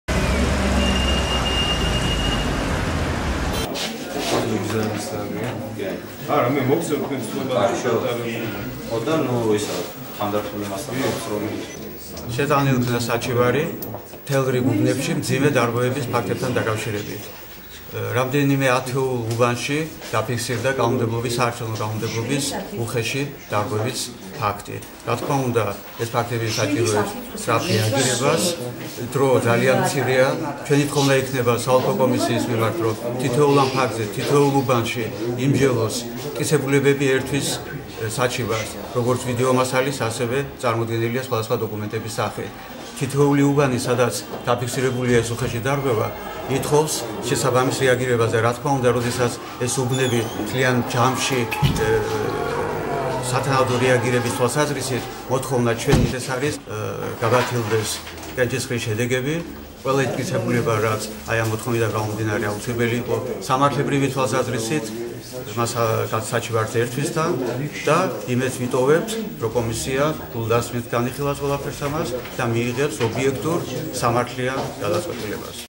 სარჩელის შეტანა
პოლიტიკური კოალიცია „ქართული ოცნების" ზუგდიდის შტაბმა  #67-ე საოლქო სარჩევნო კომისიაში საჩივრები შეიტანა. მათი თქმით, რამდენიმე ათეულ საარჩევნო უბანში დაფიქსირდა საარჩევნო კანონმდებლობის უხეში დარღვევა.„ჩვენი მოთხოვნაა გაბათილდეს არჩევნების შედეგები ზუგდიდის #67 ოლქში. დრო ძალიან მცირეა და ჩვენი თხოვნა იქნება საოლქო კომისიის მიმართ, თითოეულ ფაქტზე გულდასმით იმსჯელოს. საჩივრებს თან ერთვის ვიდეო მასალა და სხვა სახის დოკუმენტები", - განაცხადა ჟურნალისტებთან საუბრისას ბლოკ ბიძინა ივანიშვილი-ქართული ოცნების" წარმომადგენელმა ცესკოში ნუგზარ სარიამ.
 „ქართლი ოცნების" წარმომადგენლები იმედს იტოვებენ, რომ ზუგდიდის სოლქო კომისია გულდასმით განიხილავს საჩივრებს და ობიექტურ გადაწყვეტილებას მიიღებს.ხვალ, 4 ოქტომბერს კოალიციის ადგილობრივი შტაბის წევრები და მათი მხარდამჭერები ზუგდიდის #67-ე ოლქთან შეიკრიბებიან და არჩევნების შედგებს გააპროტესტებენ.